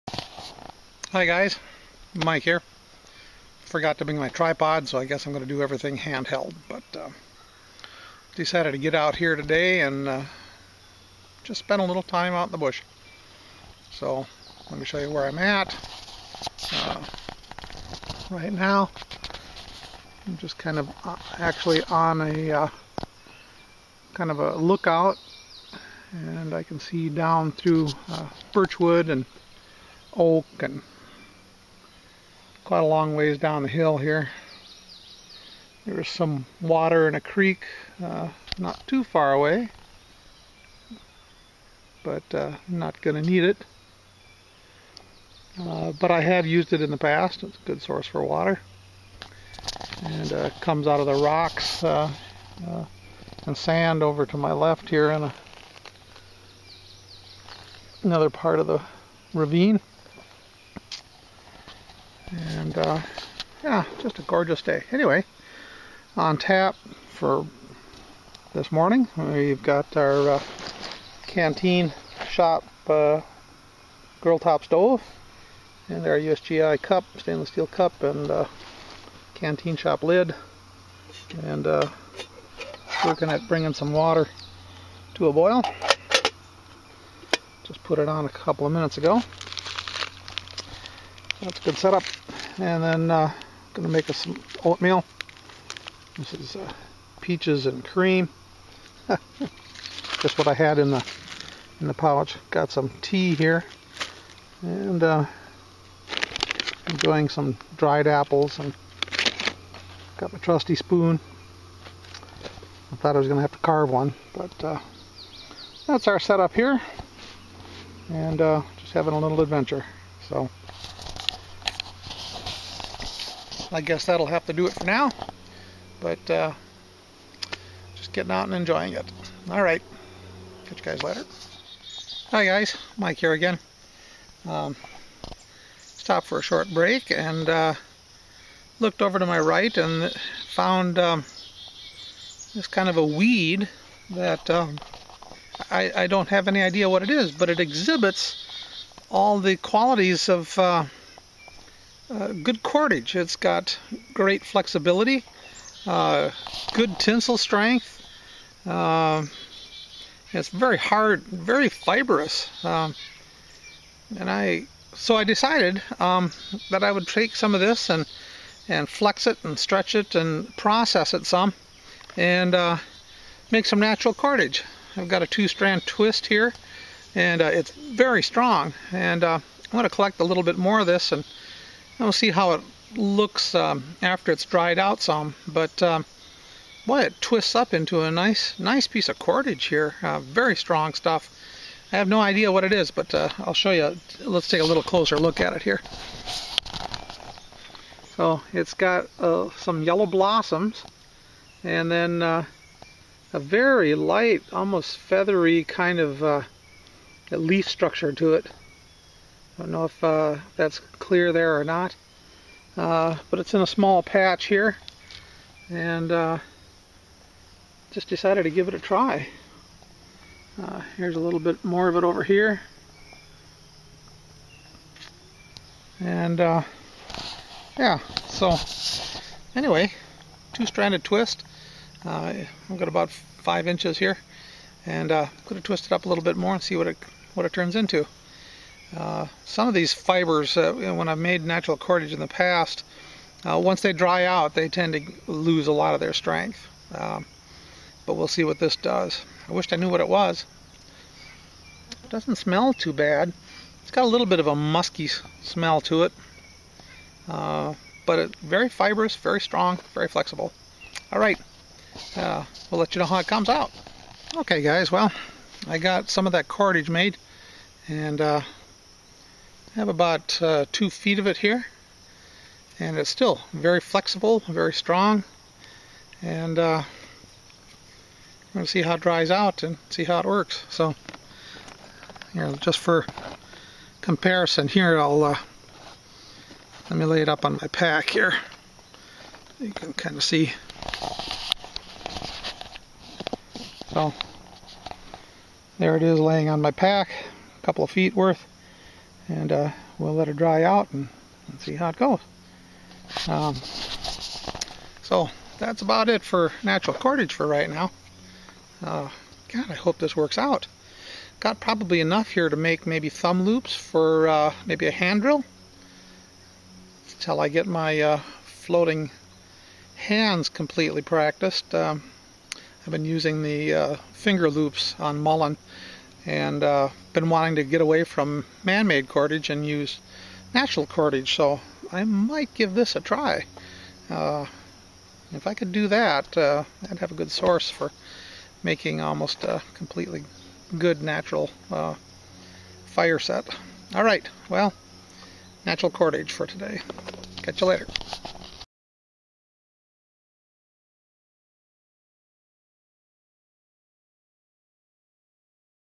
Morning Outing and Natural Cordage
Had some time, not to long ago, to get out one morning, make some breakfast with the Canteenshop Grilltop Stove, Trangia burner and my graduated USGI cup. Oatmeal, tea and dried apples were on tap and it was just the perfect morning. Later on, as I sat for a break, I noticed some weeds that exhibited the right properties for making natural cordage.
   Things to consider when selecting cordage material are the length, it's tensile strength and of course flexibility. This weed seem to exhibit all of those properties and it spun up with a two stranded twist very easily. It was a fun exercise to make something that could possibly be used for say a hand drill spindle or maybe a bow drill.
   I suspect that the cordage will lose some of it's strength after drying out, but I guess that will remain to be seen.